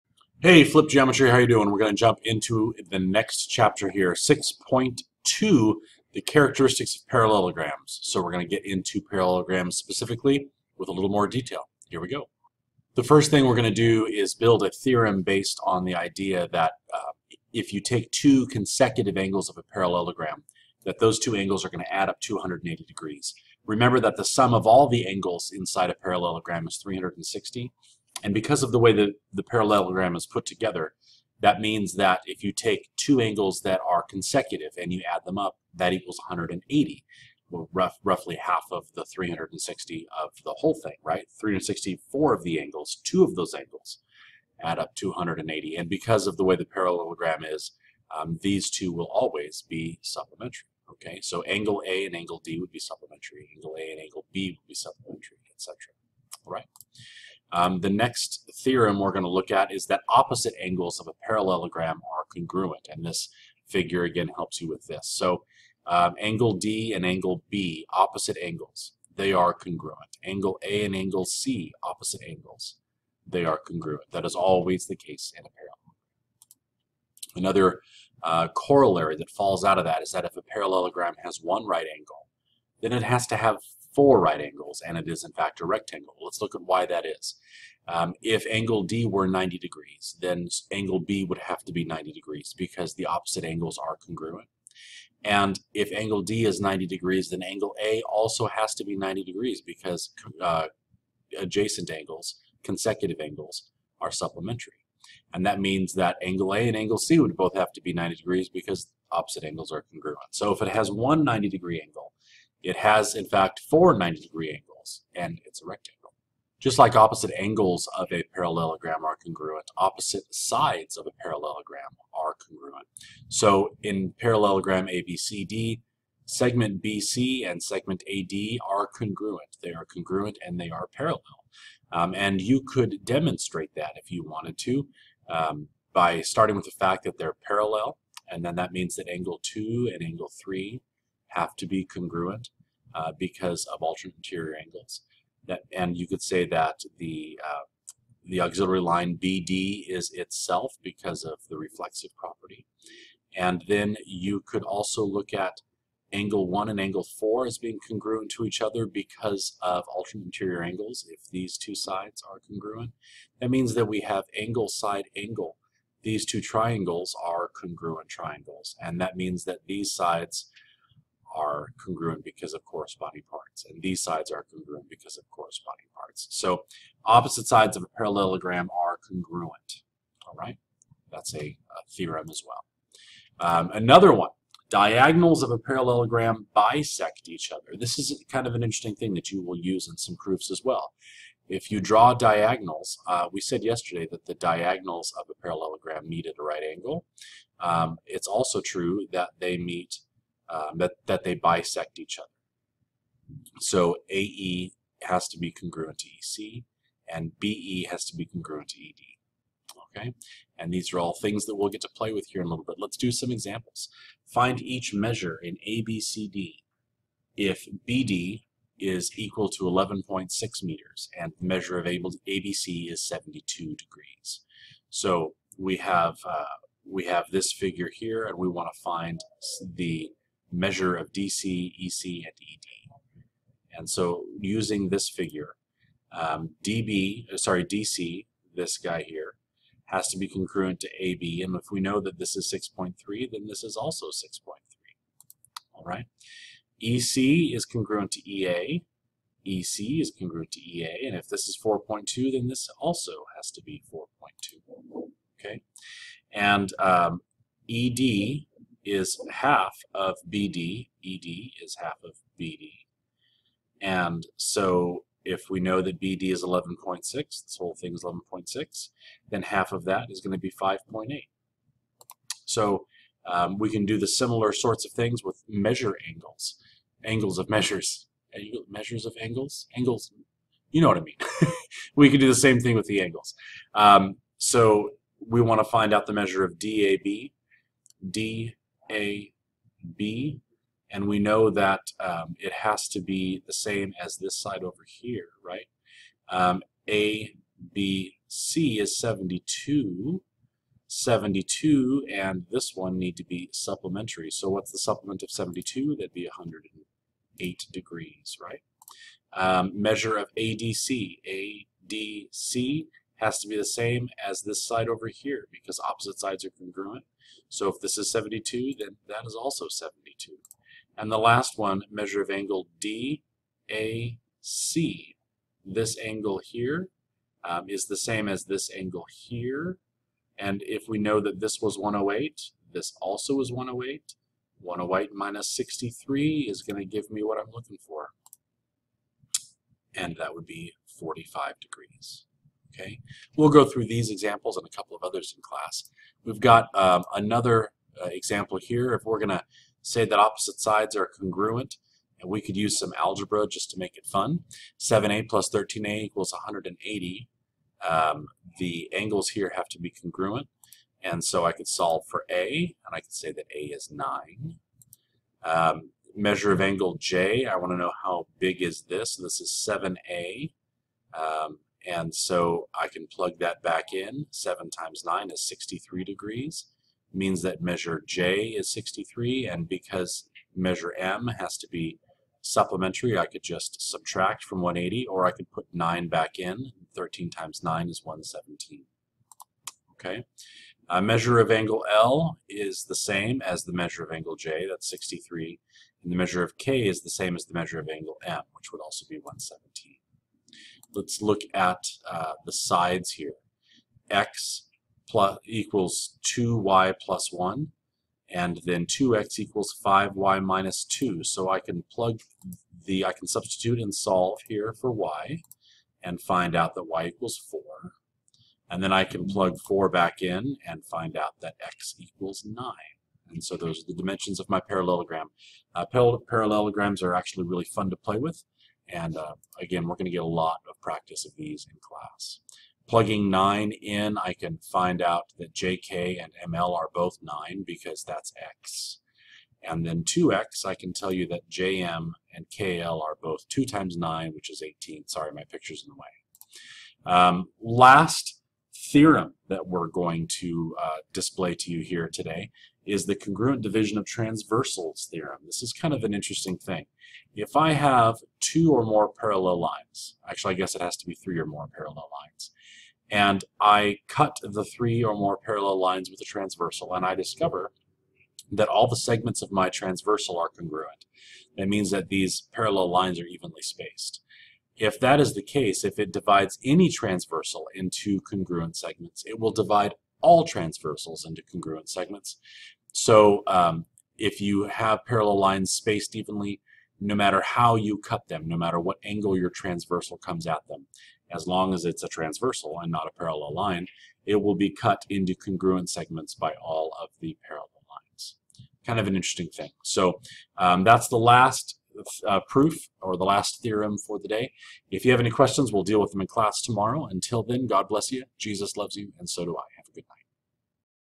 Characteristics of Parallelograms, BJU Press Geometry 4th ed Lesson 6.2--CCCS Flipped Geometry #35
This lesson explores parallelograms in detail. Consecutive angles are supplementary.  Opposite angles are congruent.  Opposite sides are congruent and supplementary.  Diagonals bisect each other.  Additionally, we look at the congruent transversal theorem for evenly spaced parallel lines.